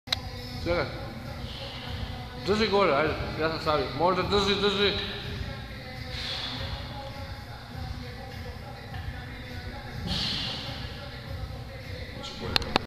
Front Lever training
A short clip from one of the front Lever sets. This is still pretty difficult for me. My bodyweight is 90kg. My record in the ideal position is about 3,5 seconds